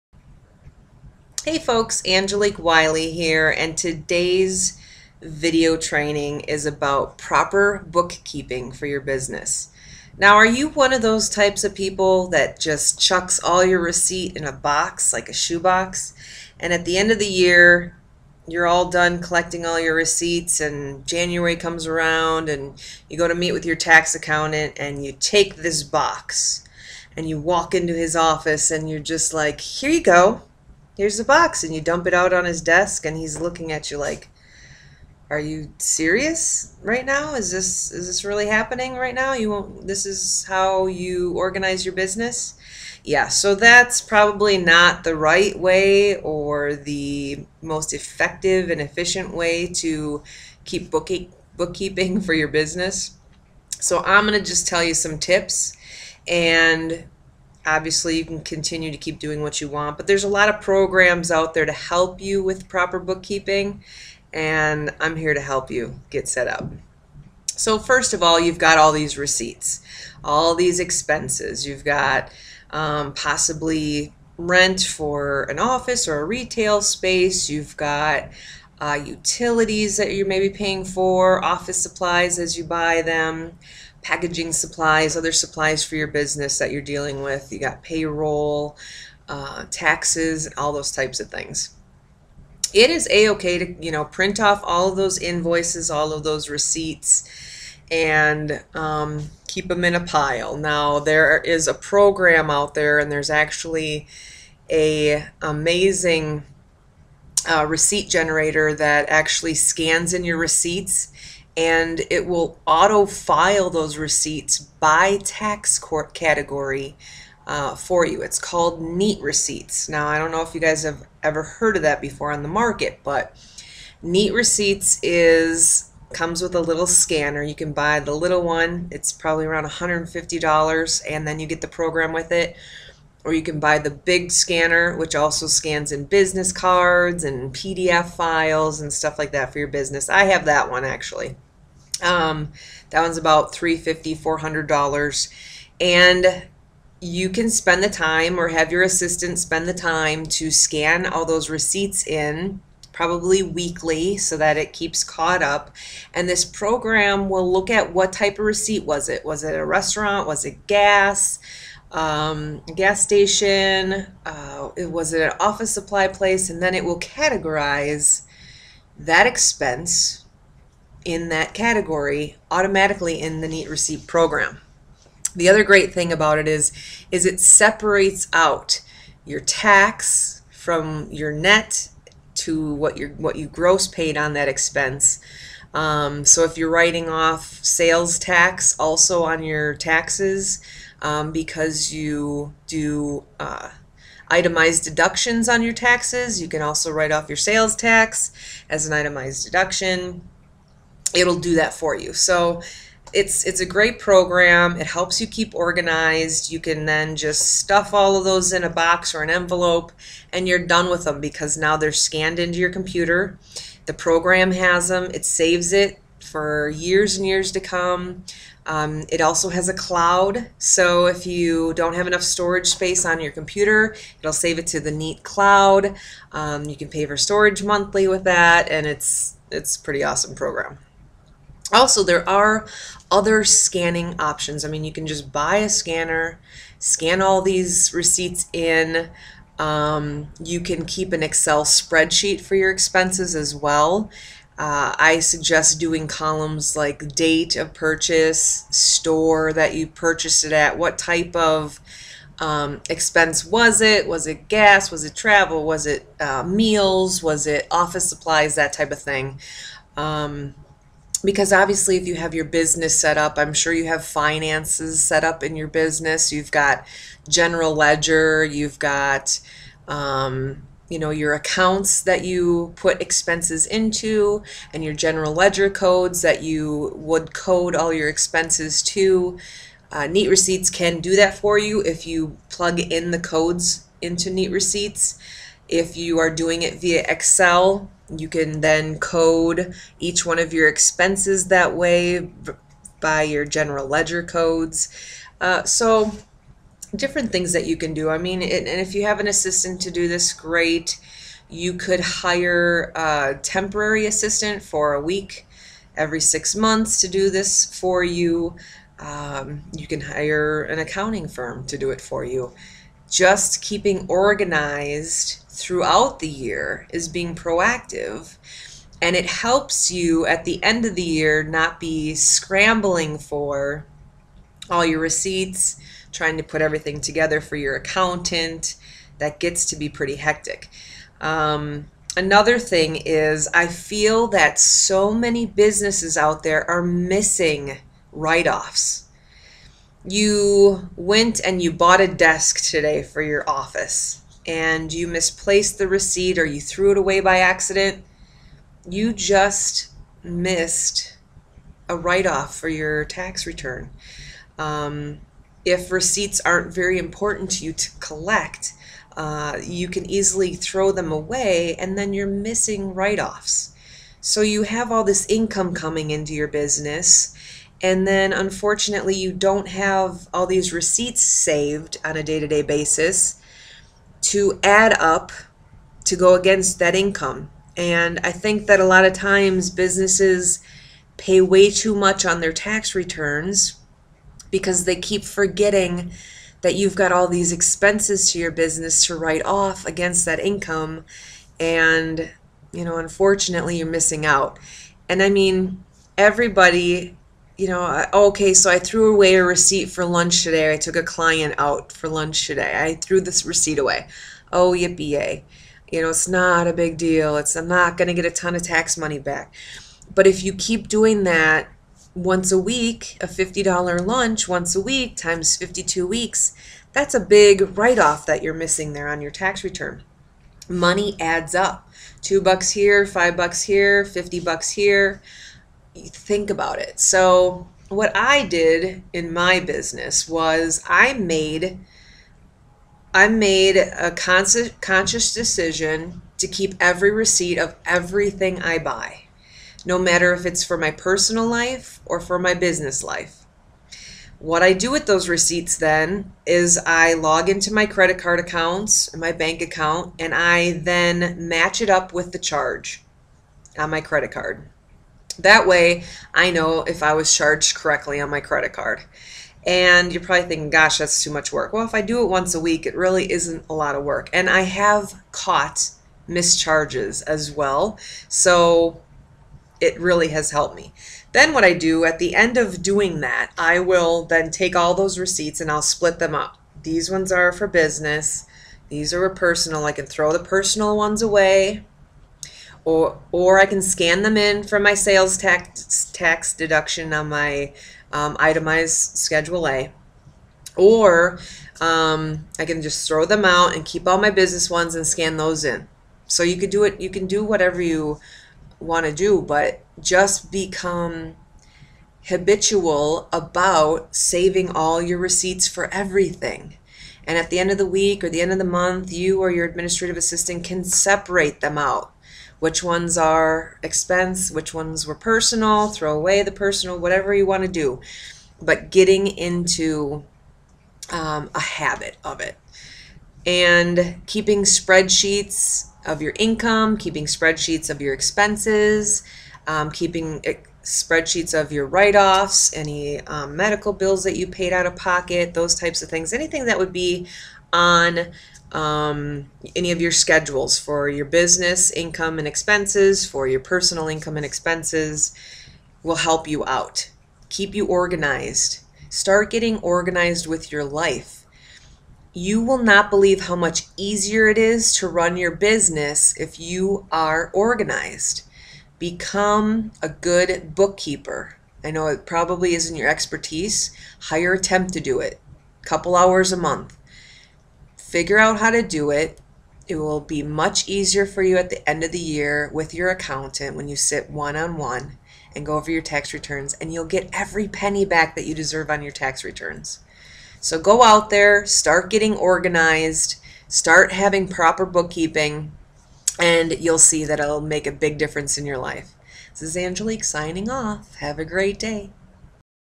Angelique Weiley - Proper Business Bookkeeping
Are you one of those business owners that take their receipts to their accountant at the end of the year in a shoebox and dumps them on your accountant's desk?  Shame on you!
Be better organized, help out your accountant!
Buy a NEAT receipts scanner and program or hire a temp to do this for you monthly in a scan program or EXCEL.
Get organized!